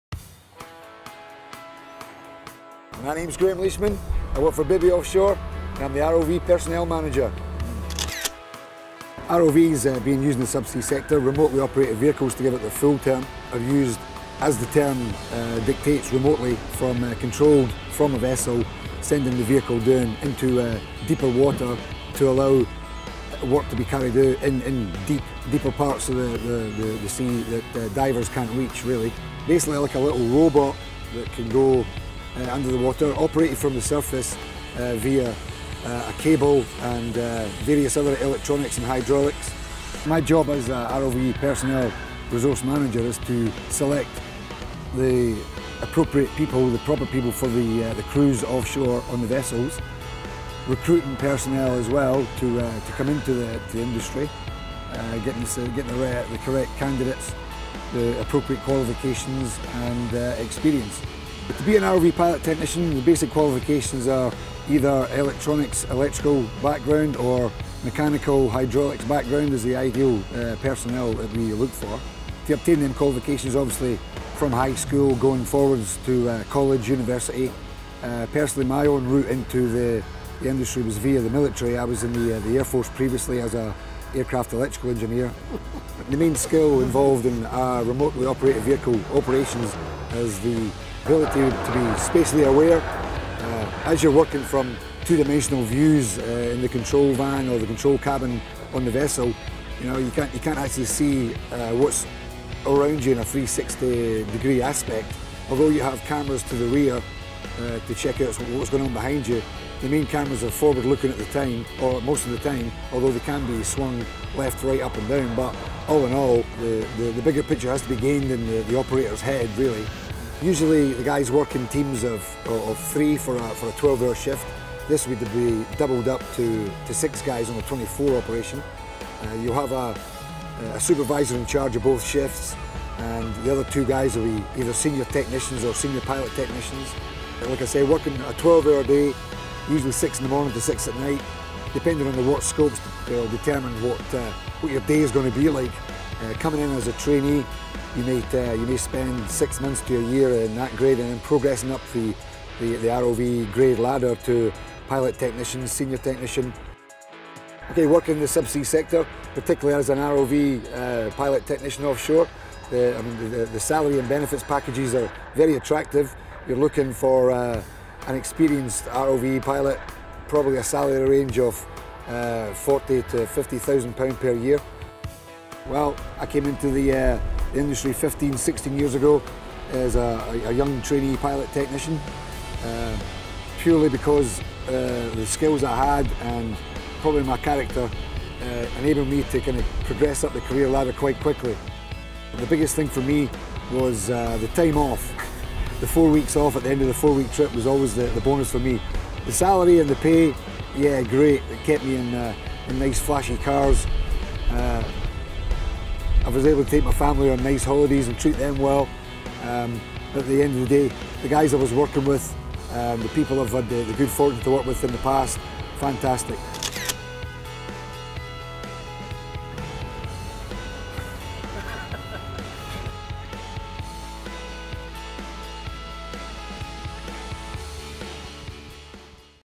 Inside Industry - Graeme - Bibby Offshore - ROV Careers
Find out what is like to work in the Energy sector using ROV's

to find out more.